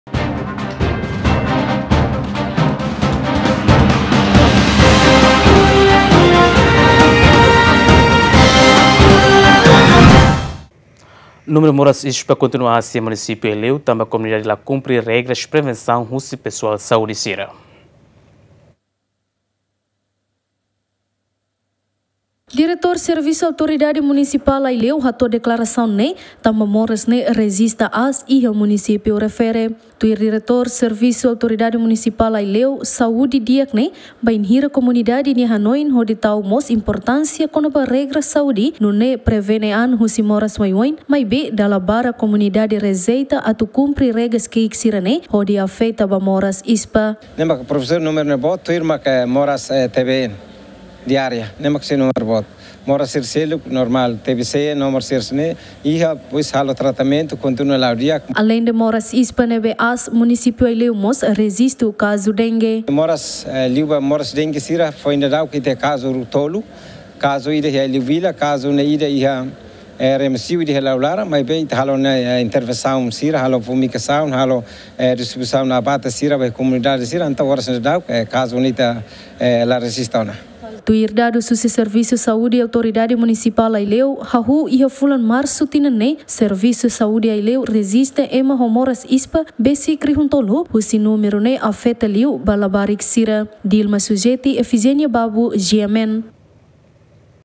Númeru moras Ispa kontinua as iha Munisípiu Aileu tanba komunidade la kumpri regras prevensaun
Númeru moras Ispa kontinua as iha Munisípiu Aileu tanba komunidade la kumpri regras prevensaun husi pesoal saúde sira